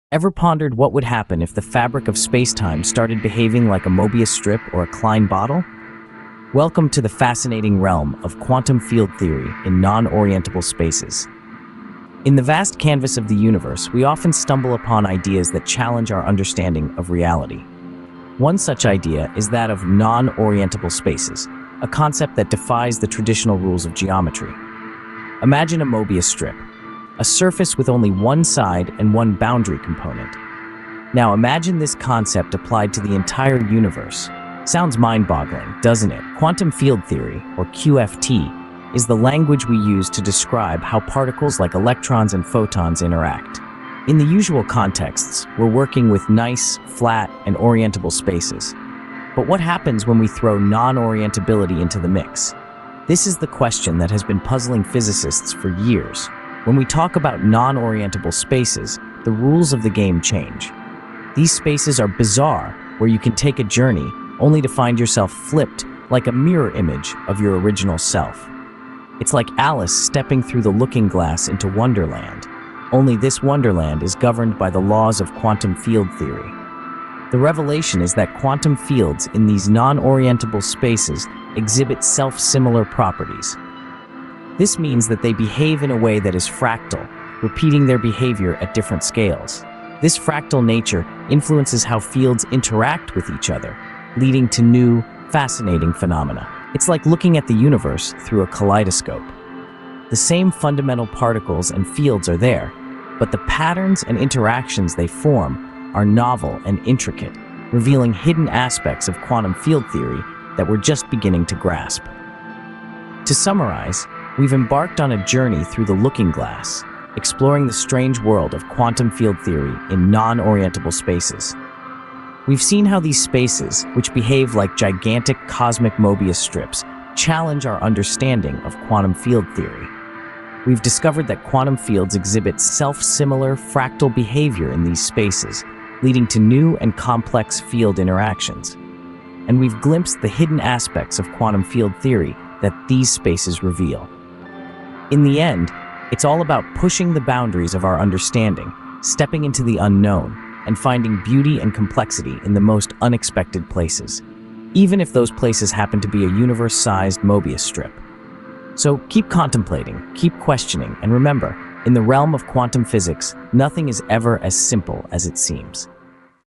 Möbius Strip of the Cosmos: Dimensions, Fractals, and Quantum Fields
Ever wondered what would happen if the fabric of space-time began to behave like a Möbius strip or a Klein bottle? In this fascinating exploration, we delve into the depths of quantum field theory (QFT) as it applies to non-orientable spaces, where traditional geometric rules no longer apply. These bizarre, mind-bending spaces challenge the very foundations of our understanding of the universe, revealing quantum fields that exhibit self-similar, fractal behavior across different scales.

In this video, we explore how non-orientable spaces, such as cosmic Möbius strips, introduce new phenomena into the interactions of particles like electrons and photons. We guide you through a kaleidoscopic journey where quantum fields behave in unexpected, yet beautiful ways, offering a glimpse into the hidden aspects of quantum field theory. The universe unfolds in astonishing patterns, with fundamental particles and fields behaving in ways we are just beginning to comprehend.

Join us as we unravel the mysteries of quantum fields in non-orientable spaces, exploring how these unique environments shape the very fabric of reality. You’ll learn how the fractal nature of these quantum fields can unlock new insights into the complexities of the universe, pushing the boundaries of physics.

If you’re intrigued by quantum physics, the Möbius strip, and fractal geometry, this video will take you on a journey through the looking glass, exploring the beauty and complexity of the universe in ways you’ve never imagined.